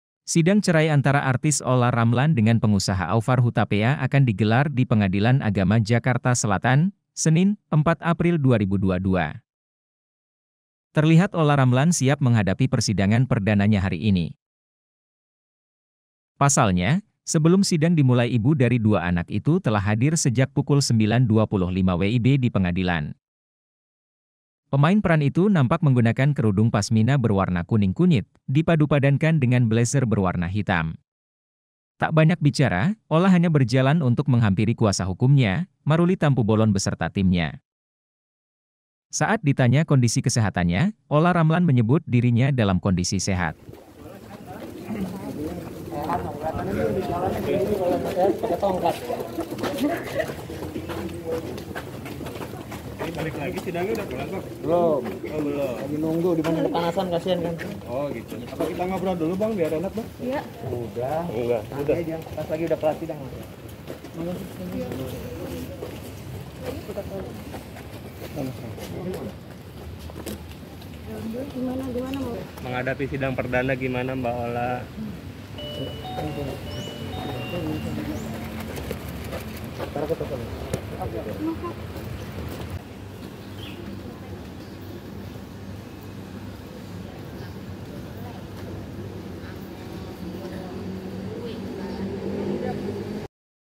Sebut Kondisinya Sehat, Olla Ramlan Hadir Pada Sidang Perdana Perceraiannya Dengan Aufar Hutapea
WARTAKOTALIVE.COM, JAKARTA -

Sidang cerai antara artis Olla Ramlan dengan pengusaha Aufar Hutapea akan digelar di Pengadilan Agama Jakarta Selatan, Senin (4/4/2022).

Terlihat Olla Ramlan siap menghadapi persidangan perdananya hari ini.

Pasalnya, sebelum sidang dimulai Ibu dari 2 anak itu telah hadir sejak pukul 09.25 WIB di pengadilan.

Pemain peran itu nampak menggunakan kerudung pasmina berwarna kuning kunyit, dipadupadankan dengan blazer berwarna hitam.

Nampak kuat lagi, Olla terlihat menggunakan high heels berwarna merah menyala, menambah keeleganannya untuk mengakhiri biduk rumah tangganya bersama dengan sang suami.